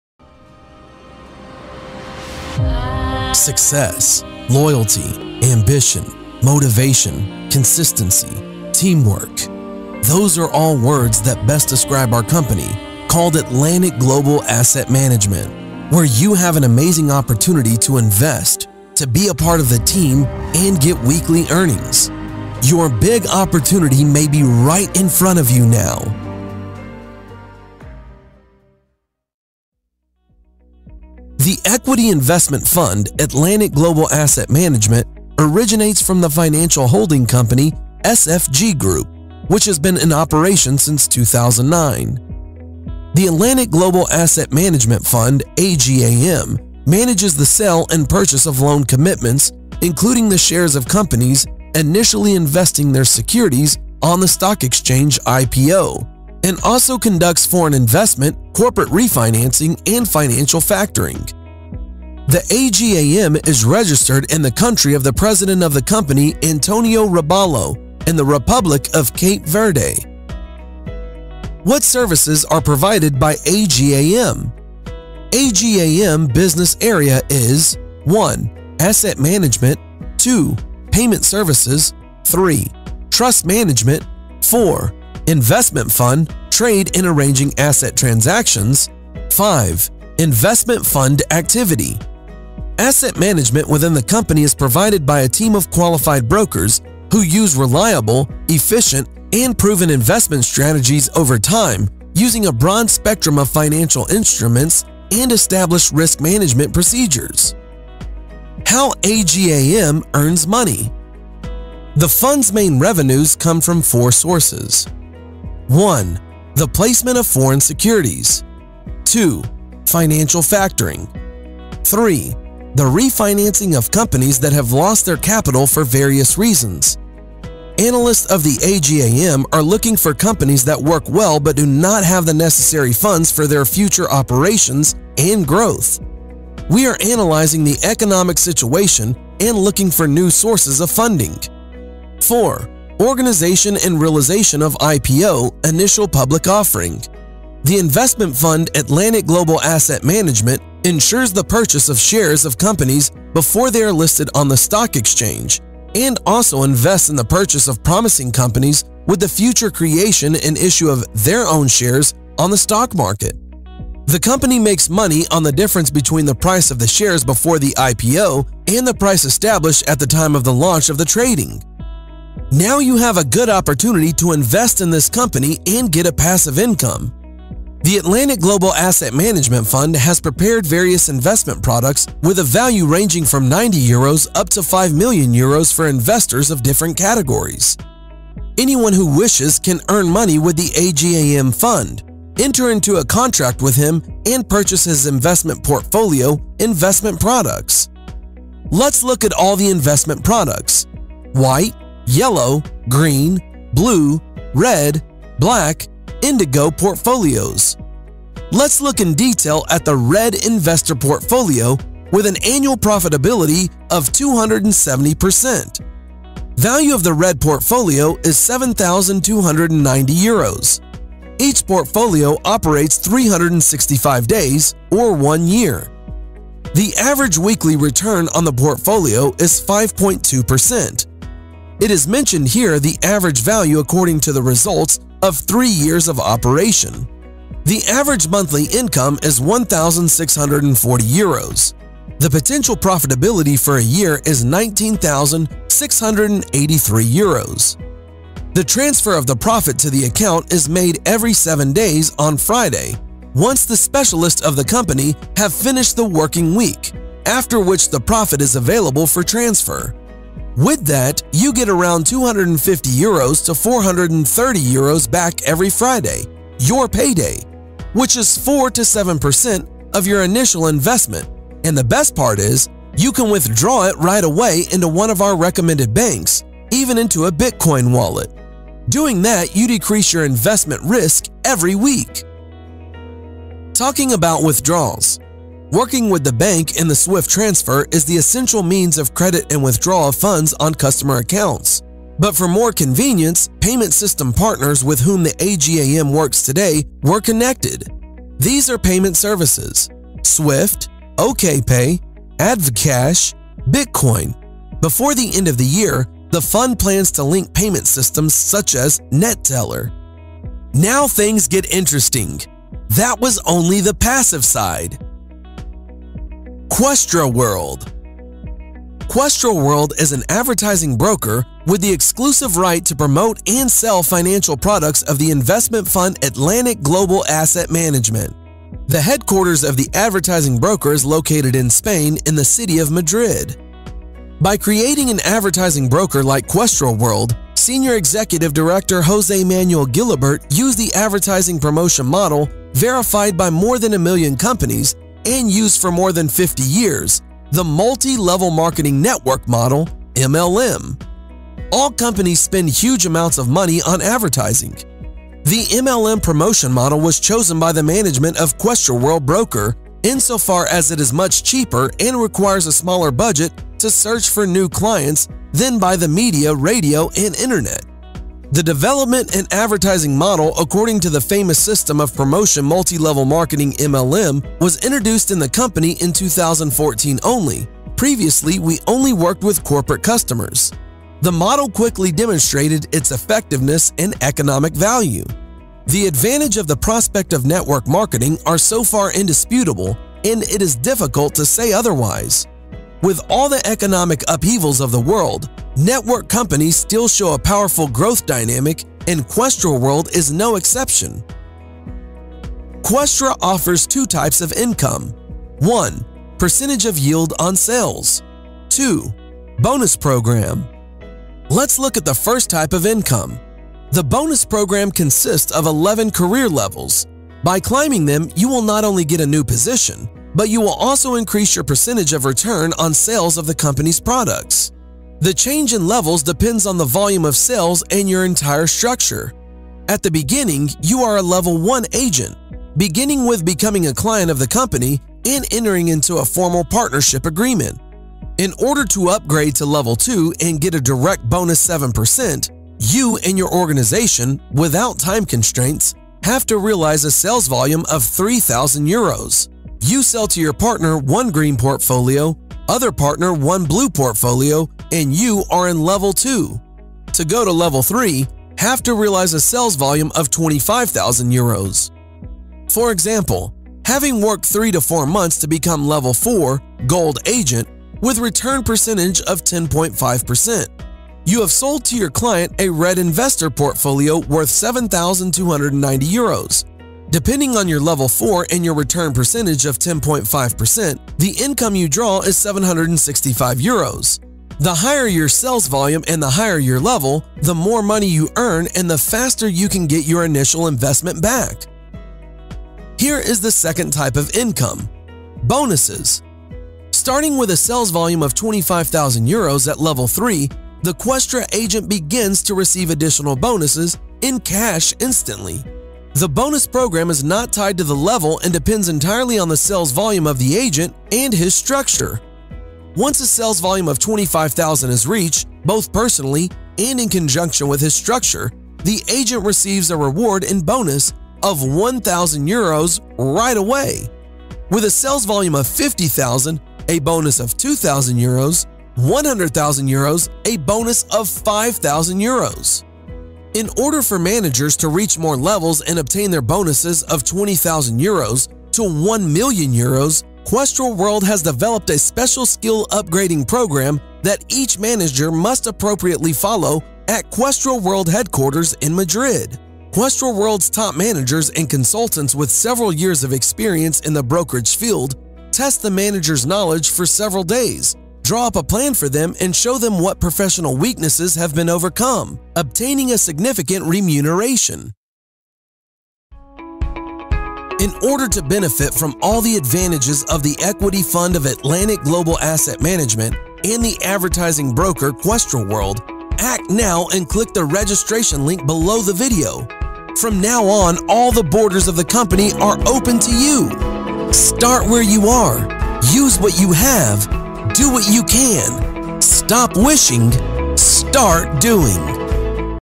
Questra World Official English Presentation 2017 Atlantic Global Asset Managem
Questra World Official English Presentation 2017 Atlantic Global Asset Management

The equity investment fund Atlantic Global Asset Management, originates from the financial holding company SFG group - Which has been in operation since 2009.

The Atlantic Global Asset Management fund A.G.A.M. manages the sale and purchase of loan commitments including the shares of companies initially investing their securities on the stock exchange IPO, and also conducts foreign investment, corporate refinancing and financial factoring.

How A.G.A.M. earns money? The funds main revenues come from 4 sources. The placement of foreign securities. Financial factoring. The refinancing of companies that have lost their capital for various reasons. Analysts of the A.G.A.M.  Are looking for companies that work well but do not have the necessary funds for their future operations and growth.  We are analysing the economic situation and looking for new sources of funding. The company makes money on the difference between the price of the shares before the IPO, and the price established at the time of the launch of the trading. Now you have a good opportunity to invest in this company and get a passive income.

The Atlantic Global Asset Management fund has prepared various investment products with a value ranging from 90 euros up to 5 million euros for investors of different categories. Anyone who wishes can earn money with the A.G.A.M. fund - Enter into a contract with him, and purchases investment portfolio investment products. Let's look in detail at the Red investor portfolio with an annual profitability of 270%. Value of the Red Portfolio is 7,290 euros.  Each portfolio operates 365 days or one year.

The average weekly return on the portfolio is 5.2%.  It is mentioned here the average value according to the results of three years of operation.  The average monthly income is 1,640 euros. The potential profitability for a year is 19,683 euros.  The transfer of the profit to the account is made every 7 days on Friday, once the specialist of the company has finished the working week after which the profit is available for transfer.

With that, you get around 250 euros to 430 euros back every Friday – “Your pay day”, which is 4 – 7% of your initial investment, and the best part is you can withdraw it right away into one of our recommended banks - even into a Bitcoin wallet! Doing that, you decrease your investment risk every week.

Talking about withdrawals, working with the bank in the Swift transfer is the essential means of credit and withdraws funds on customer accounts, but for more convenience payment system partners with whom the A.G.A.M. works today were connected.   Now things get interesting that was only the passive side. Questra World is an advertising broker with the exclusive right to promote and sell financial products of the investment fund, Atlantic Global Asset Management.  The headquarters of the advertising broker is located in Spain in the city of Madrid.  By creating an advertising broker like Questra World, senior executive director Jose Manuel Gilabert used the advertising promotion model verified by more than a million companies and used for more than 50 years the multilevel marketing network model MLM.

The development and advertising model according to the famous system of promotion multi-level marketing MLM was introduced in the company in 2014 only. Previously we only worked with corporate customers. The model quickly demonstrated its effectiveness in economic value. The advantage of the prospect of network marketing is so far indisputable, and it is difficult to say otherwise. Questra offers two types of income. Percentage of yield on sales. Bonus program Let's look at the first type of income the bonus program consists of 11 career levels. By climbing them you will not only get a new position, but you will also increase your percentage of return on sales of the company's products. Here is the 2nd type of income; Bonuses Starting with a sales volume of 25,000 euros at Level 3, the Questra agent begins to receive additional bonuses in cash instantly.

In order to benefit from all the advantages of the equity fund of Atlantic Global Asset Management in the advertising broker Questra World, act now and click the registration link below the video from now on all the borders of the company are open to you! With best regards Questra World.